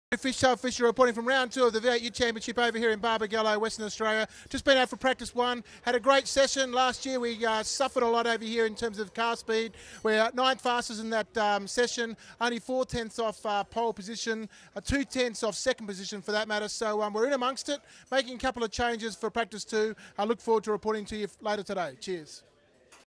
Practice 1 Perth400 2014 - Jesus Racing - Andrew Fishtail Fisher
Fishtail reports in from Barbagallo Raceway after finishing 9th fastest in practice 1!